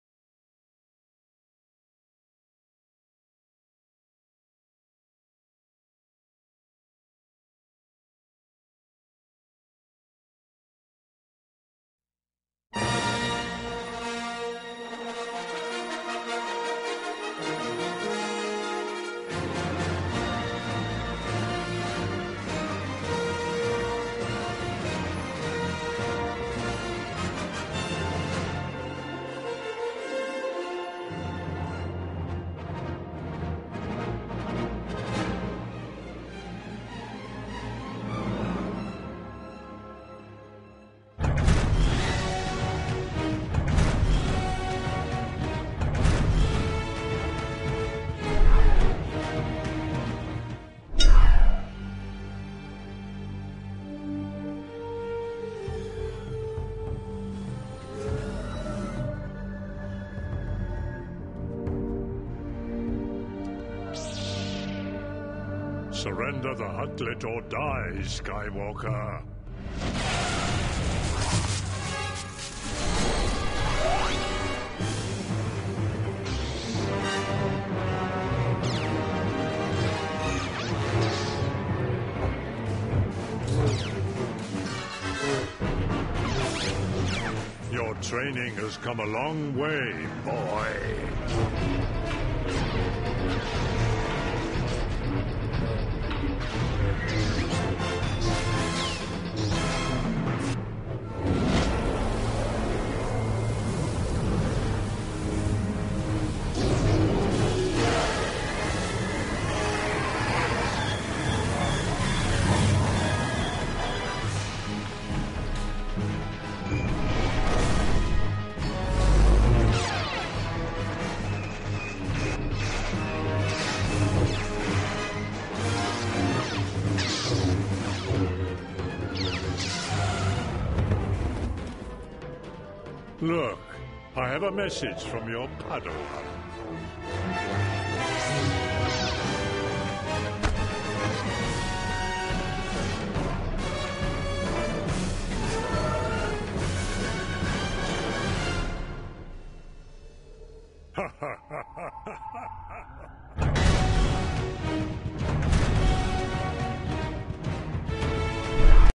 Star Wars Lightsaber Battle 星球大戰光劍對戰
Star Wars Lightsaber Battle - Battle 1  "Anakin Skywalker vs General Grievious" 星球大戰光劍對戰  第1戰役
Check out more Star Wars items at Hong Kong Toys RUs